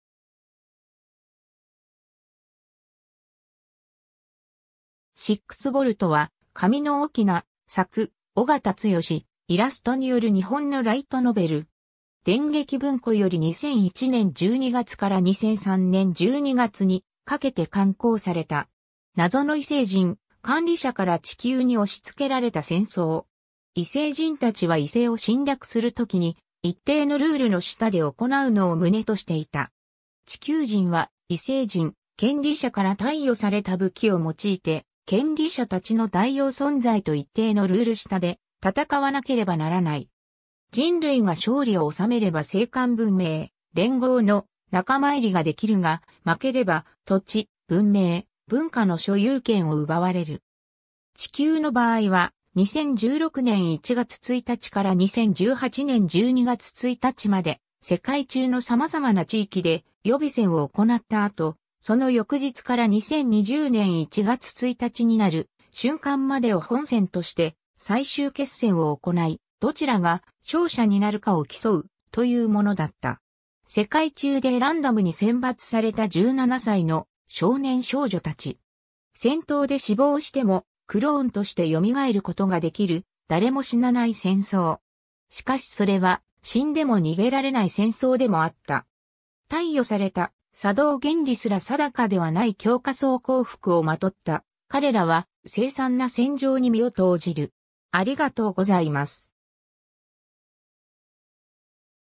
シックス・ボルト
『シックス・ボルト』は、神野オキナ/作・緒方剛志/イラストによる日本のライトノベル。
電撃文庫（メディアワークス）より2001年12月から2003年12月にかけて刊行された。
 謎の異星人「管理者」から地球に押し付けられた戦争。
異星人たちは異星を侵略する時に一定のルールの下で行うのを旨としていた。
地球人は異星人「権利者」から貸与された武器を用いて「権利者」たちの代用存在と一定のルール下で戦わなければならない。
人類が勝利をおさめれば星間文明「連合」の仲間入りができるが、負ければ「土地、文明、文化の所有権を奪われる」。
地球の場合は、2016年1月1日から2018年12月1日まで世界中の様々な地域で予備戦を行った後、その翌日から2020年1月1日になる瞬間までを本戦として最終決戦を行い、どちらが勝者になるかを競う、というものだった。
 世界中でランダムに選抜された17歳の少年少女たち。
戦闘で死亡してもクローンとして蘇ることができる「誰も死なない」戦争。
しかしそれは「死んでも逃げられない」戦争でもあった。
貸与された作動原理すら定かではない強化装甲服をまとった彼らは凄惨な戦場に身を投じる。